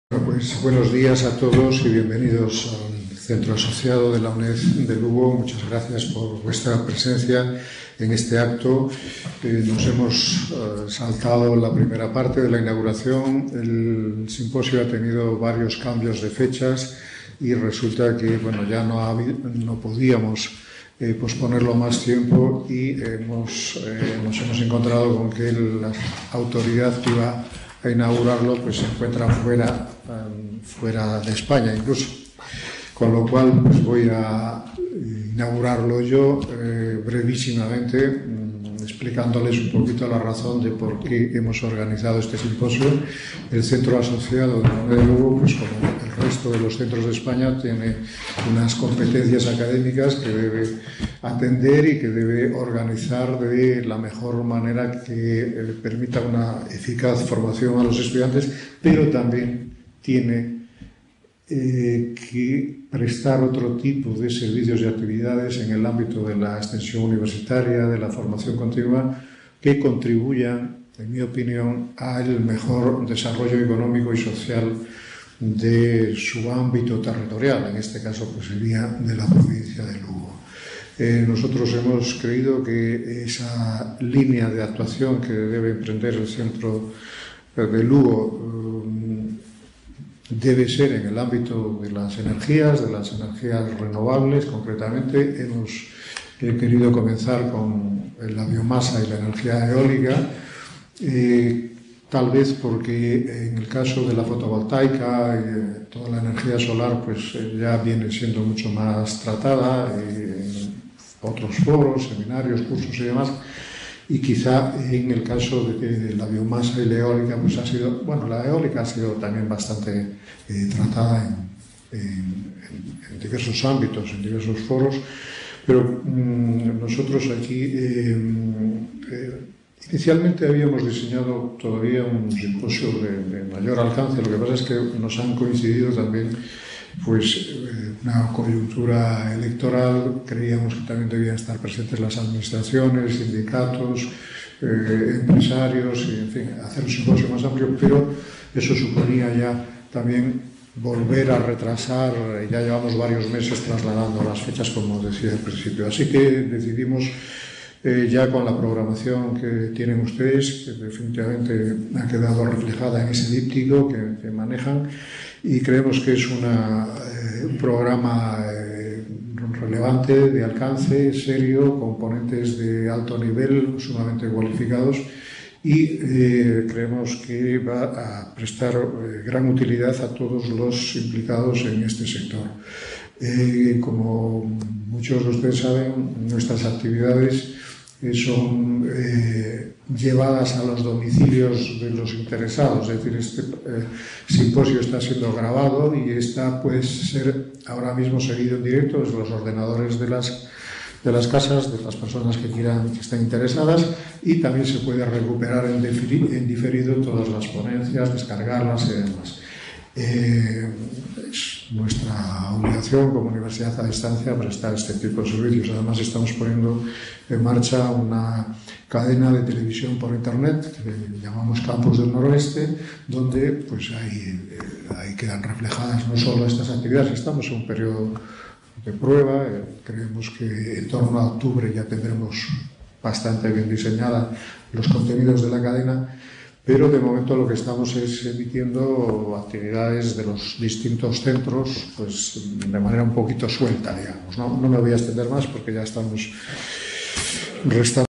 Simposio Recursos energéticos de la biomasa y el viento. Inauguración. 07-05-2009.
Simposio Recursos energéticos de la biomasa y el viento

Información: En este conjunto de videoconferencias se explican las claves de un desarrollo económico regional a través del uso de energías renovables y respetuosas con el medio ambiente como son la biomasa y la energía eólica.

Director:José Luis Prieto Arroyo
Director del Centro Asociado de la UNED de Lugo.
Coordinador: Rogelio Ordóñez Blanco,
Director de INORVI
Fechas: 7 y 8 de mayo de 2009.
Lugar: Salón de Actos Centro Asociado de la UNED Lugo
Rúa Cantigas e Frores, 31
Campus Universitario Lugo